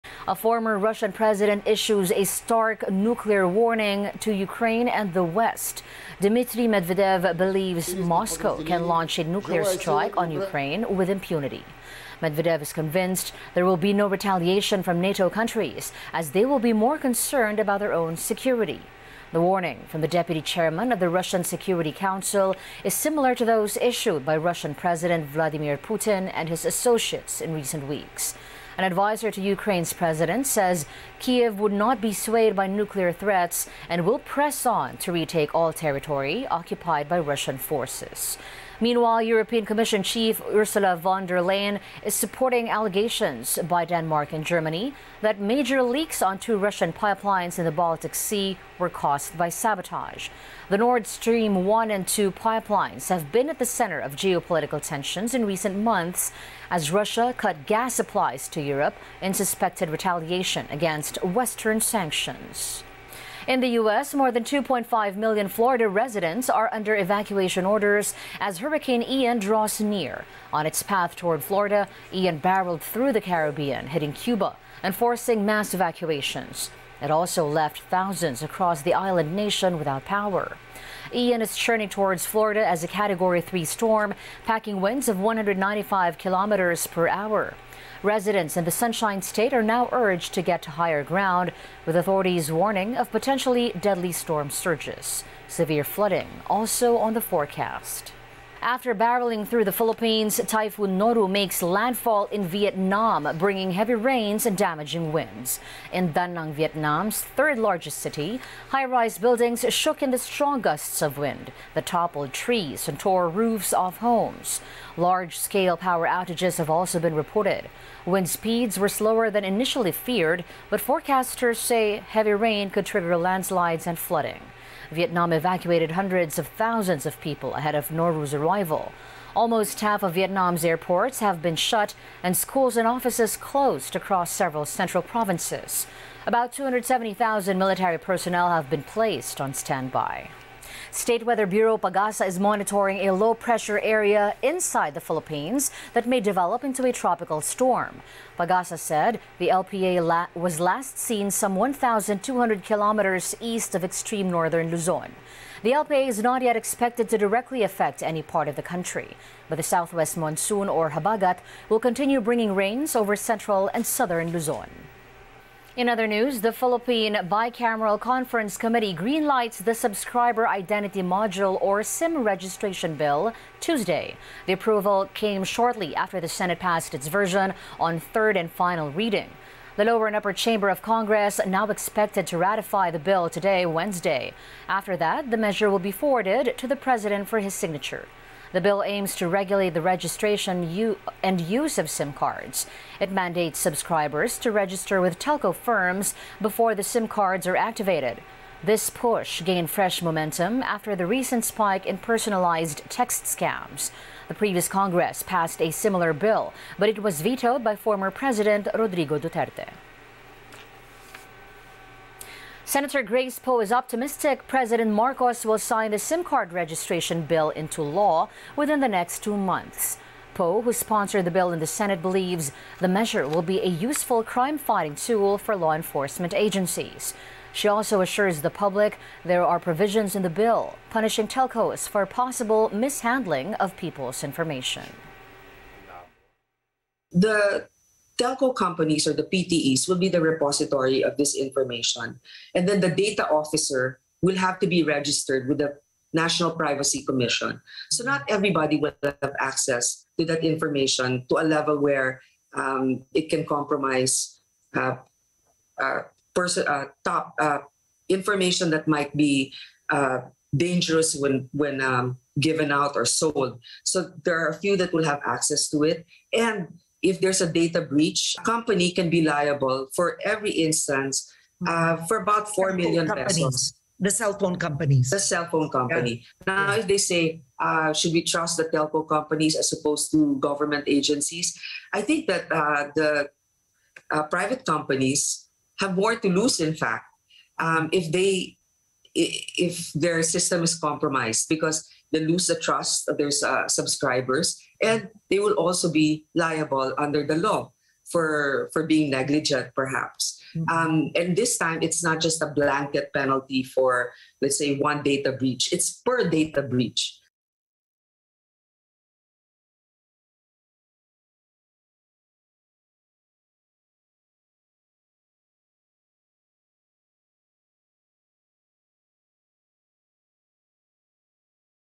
Putin nuclear threat 'not a bluff,' says former Russian President | ANC
Rundown: A former Russian president issues a stark nuclear warning to Ukraine and the West.